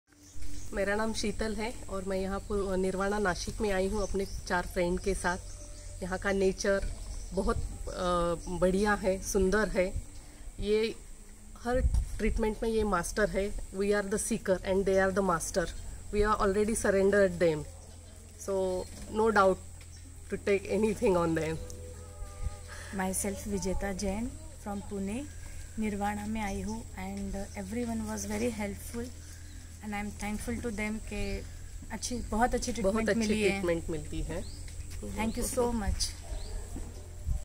Guests Reviews about our Naturopathic Treatments!
Take a look at what guests are saying about our Naturopathic_Treatments!
We are open now with all precautionary_measures and protocols to ensure your health_and_safety.
✅ Sanitizing Entire Facility Frequently
✅ Using Face Masks & Hand Sanitizers
✅ Maintaining Social Distancing
✅ Monitoring Our Staff’s Health
✅ Managing Guest Occupancy Rate
Adopt Naturopathy, Boost_immunity & Stay_Safe.